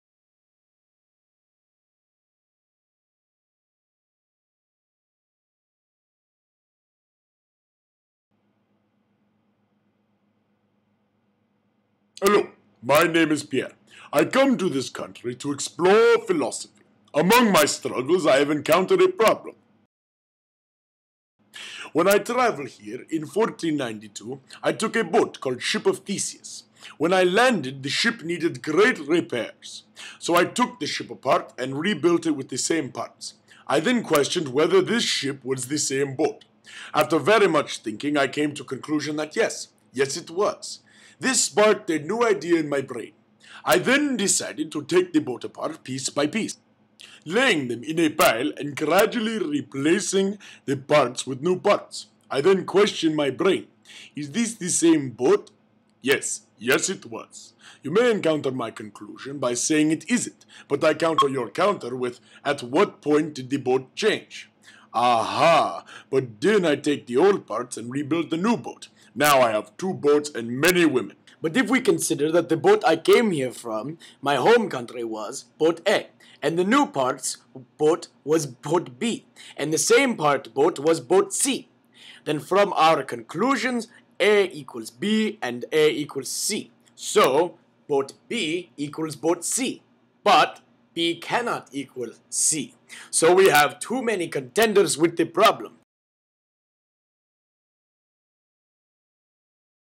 Ship of Theseus
A heart touching monologue by the wandering philosopher named Pierre.